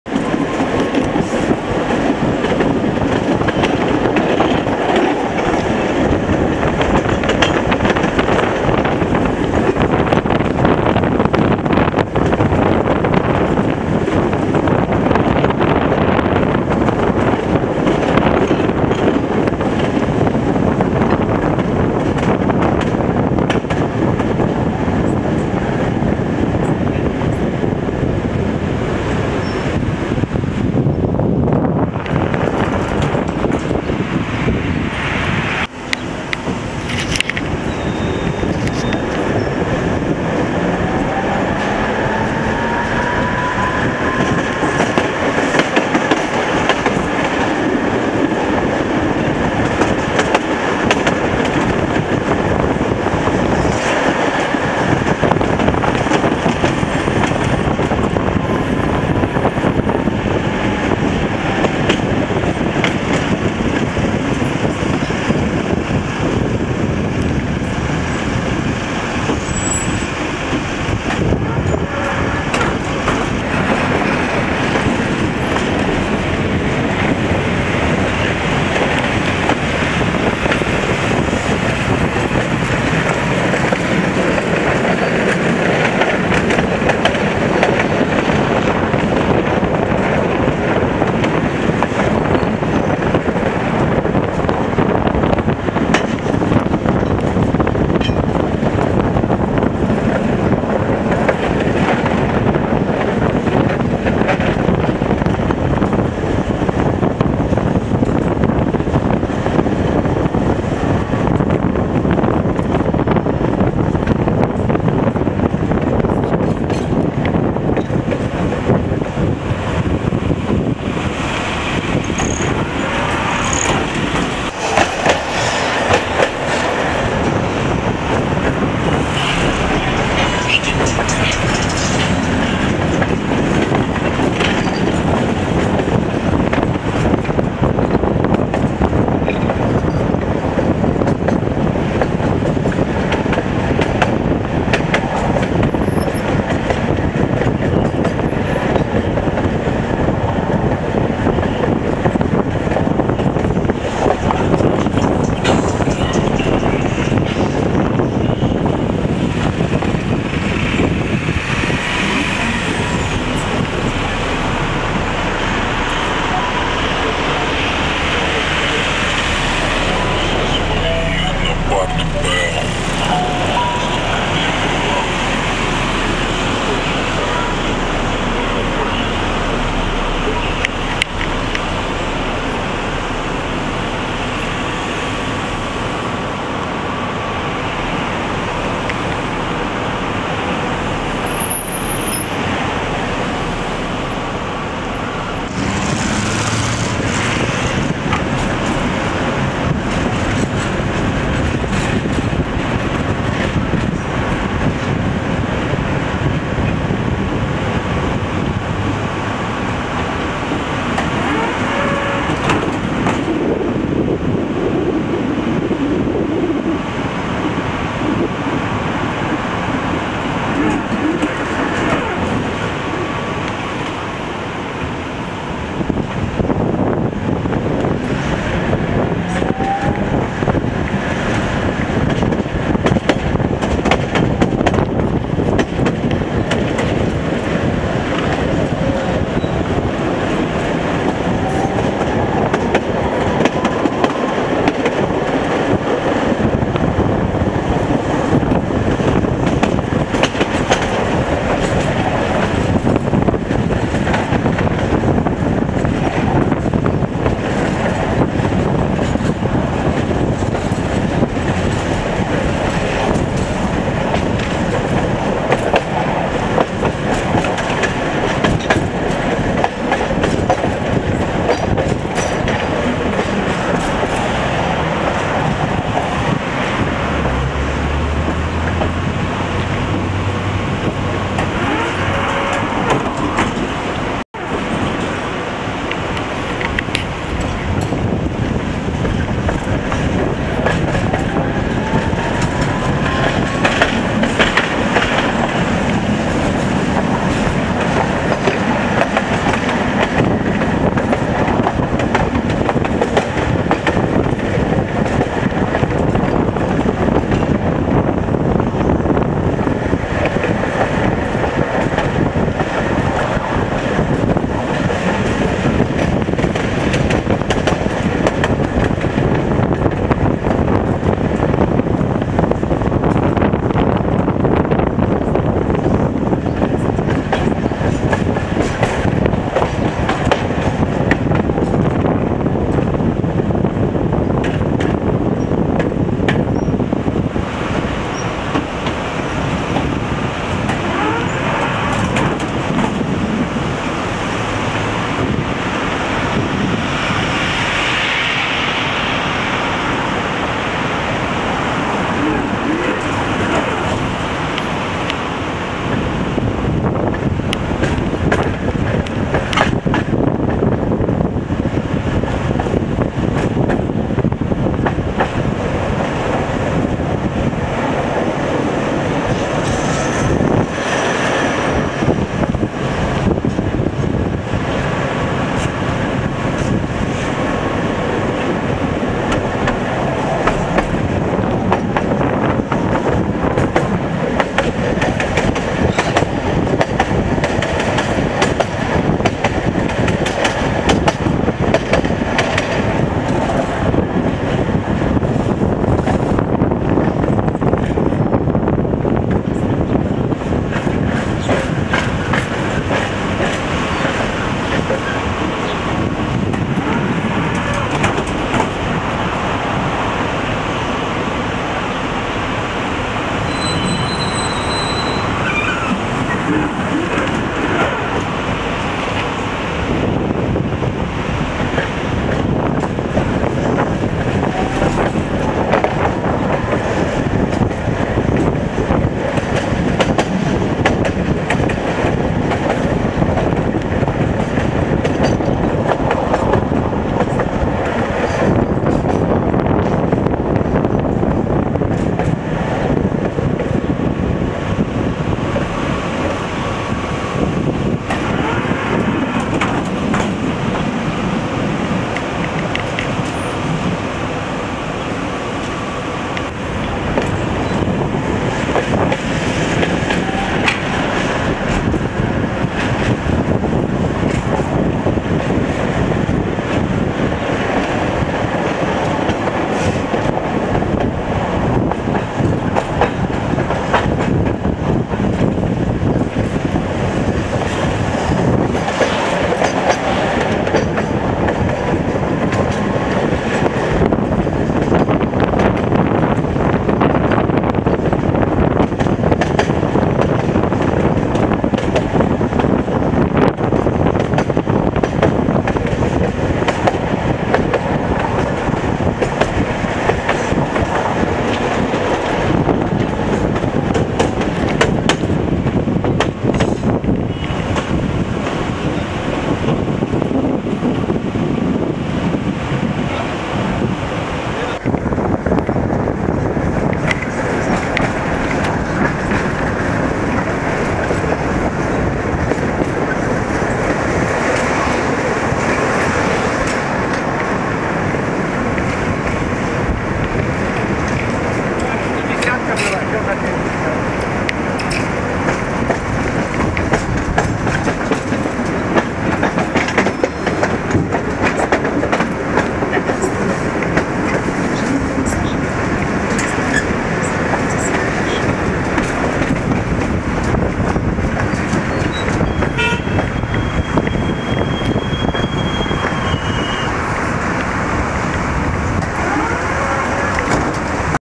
Поездка на трамвае от остановки Дружининская до Таганского Ряда
Tramway trip in Ekaterinburg from Druzhininskaya stop to "Tagansky Ryad" market. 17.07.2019.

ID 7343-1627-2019